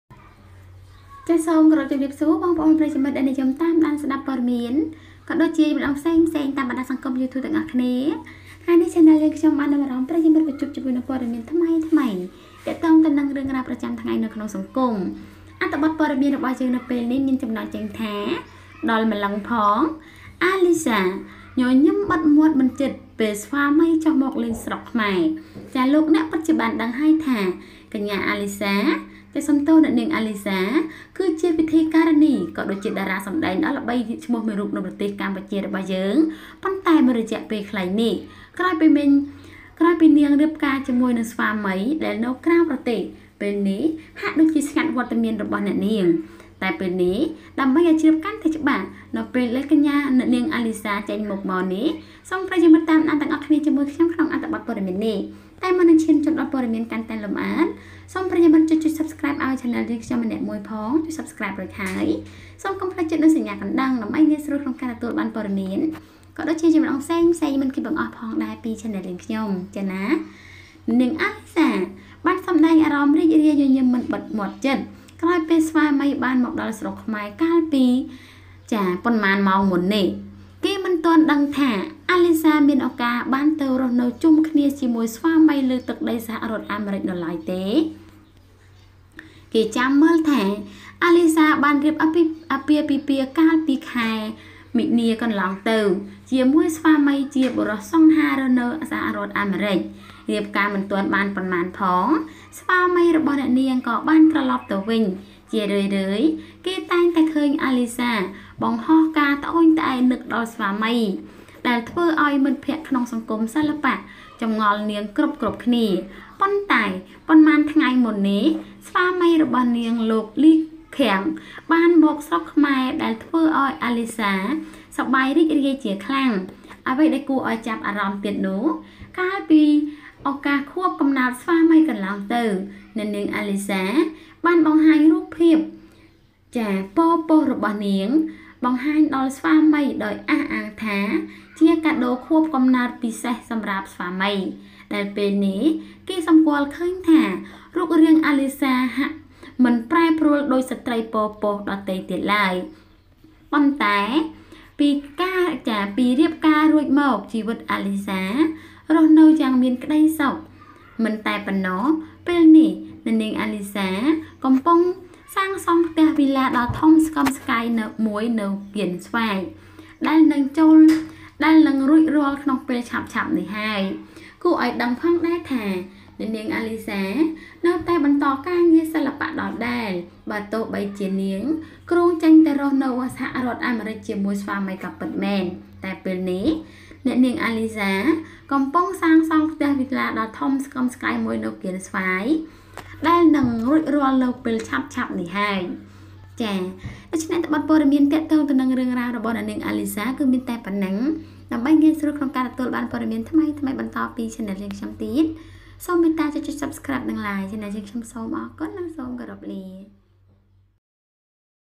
ដល់ម្លឹងផង!!!​ អា​លី​ហ្សា​ ​ញញឹម​បិទមាត់​មិន​ជិត​ពេល​ស្វាមី​ចុះ​មកលេង​ស្រុកខ្មែរ​
ដល់ម្លឹងផង!!!​ អា​លី​ហ្សា​ ​ញញឹម​បិទមាត់​មិន​ជិត​ពេល​ស្វាមី​ចុះ​មកលេង​ស្រុកខ្មែរ​ Also Alicia smiled silently when her husband visited Cambodia.